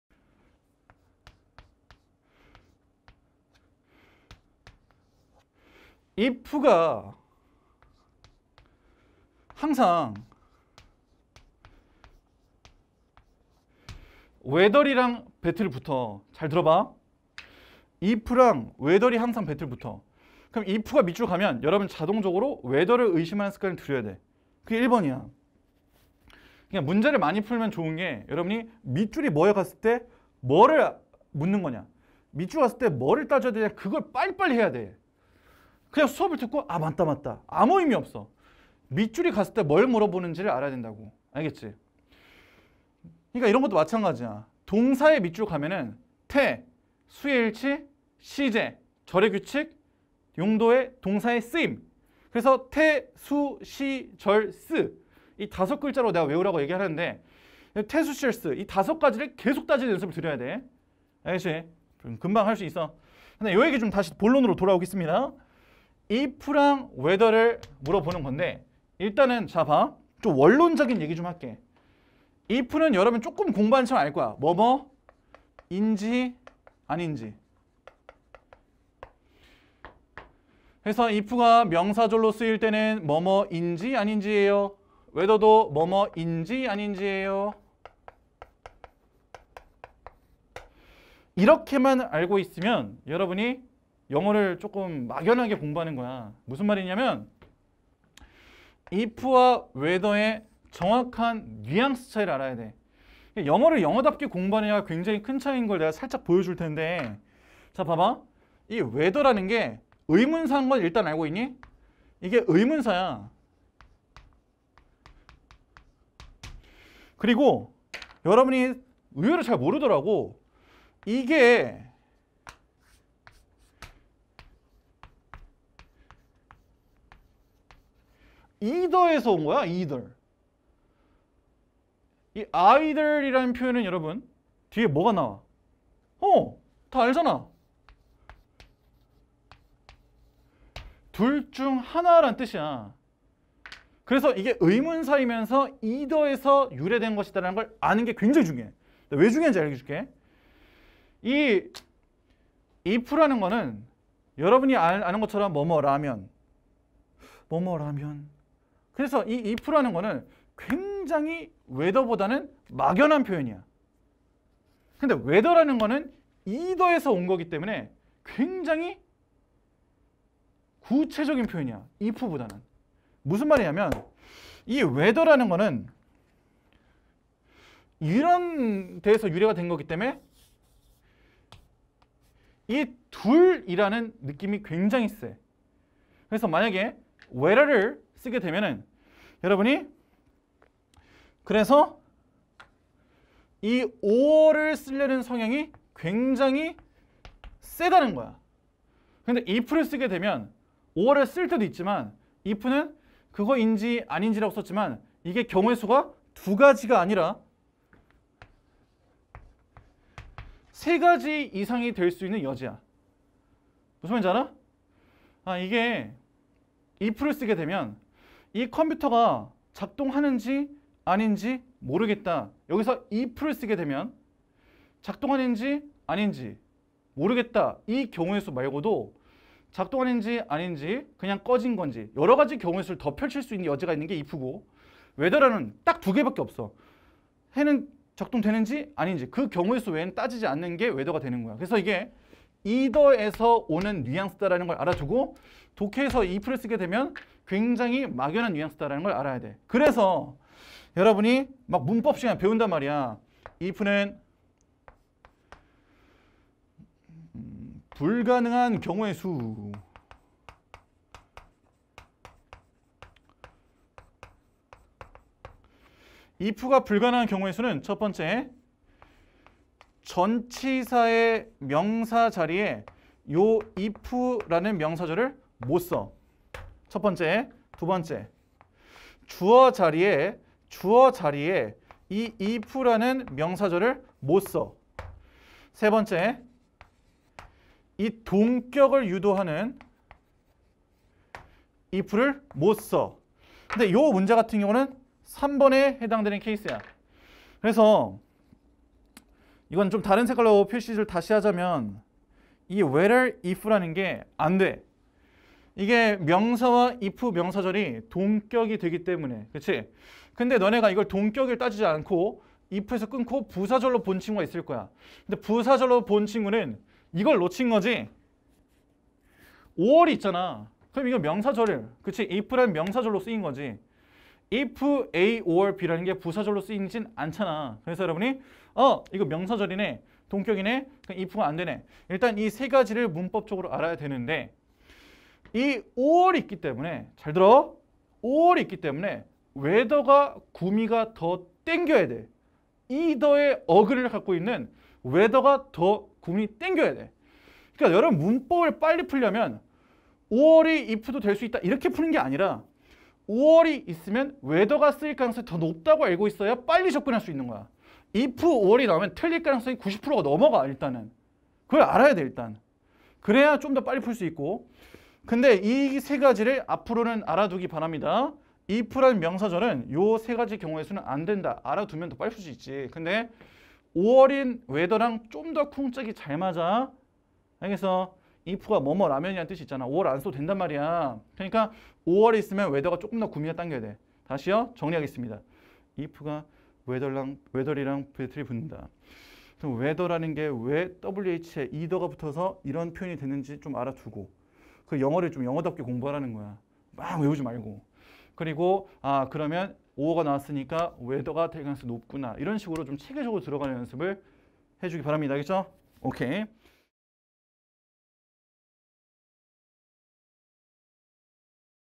[다니엘영어] if와 whether 차이!? 영어는 영어답게 공부하자!!
영어는 영어답게!  if와 whether 차이! 이제 간단하게 구분하자!